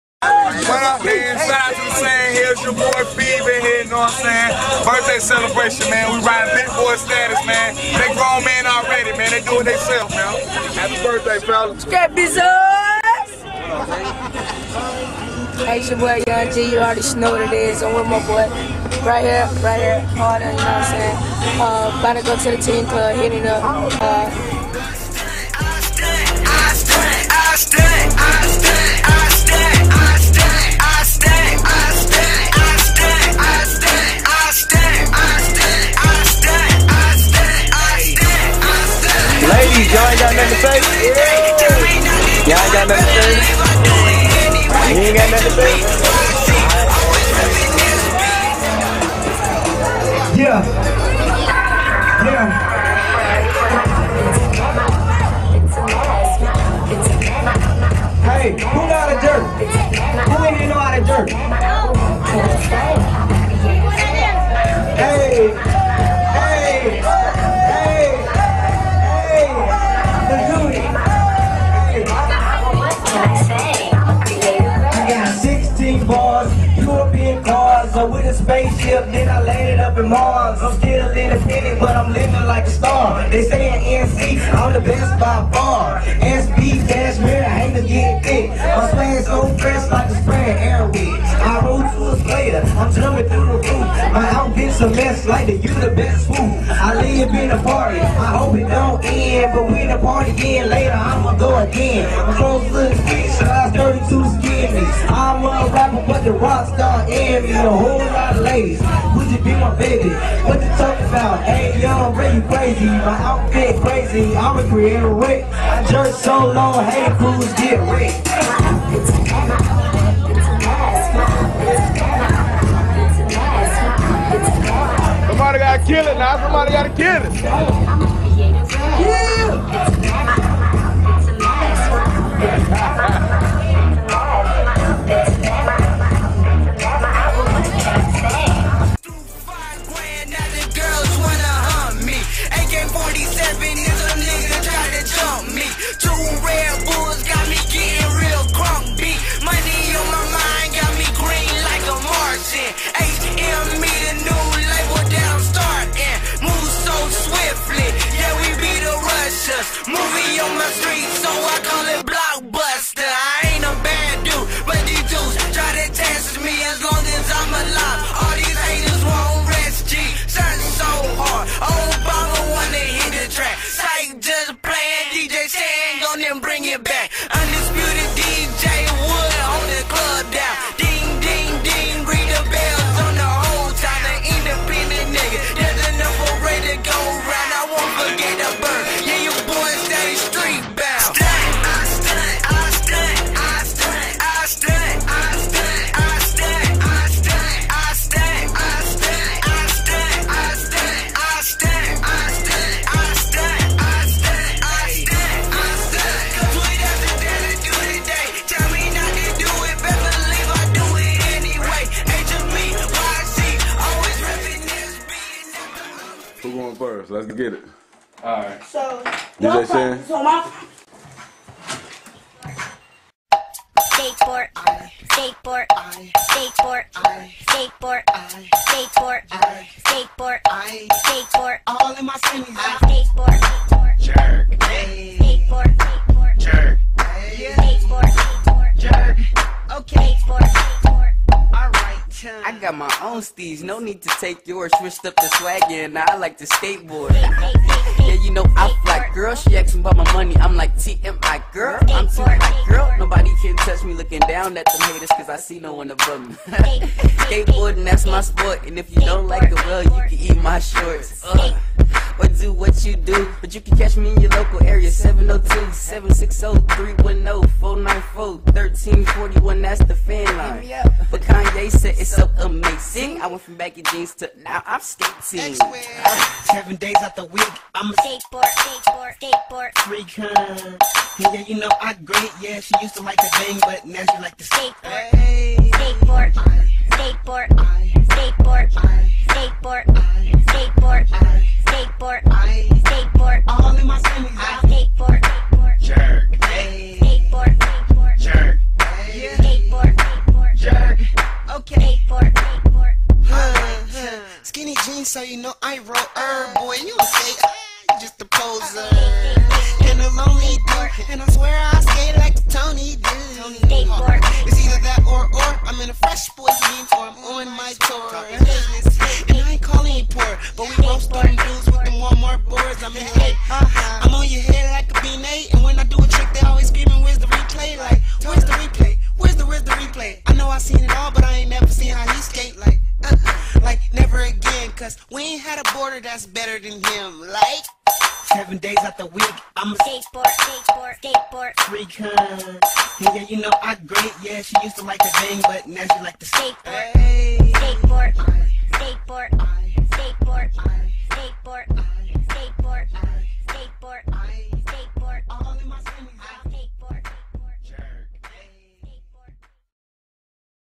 Scrap Boyz at Club Showtime
Super old!! Performance we did at Young Gee and D-tang's birthday bash, We was goin off. Plus a lil jerkin at the end lol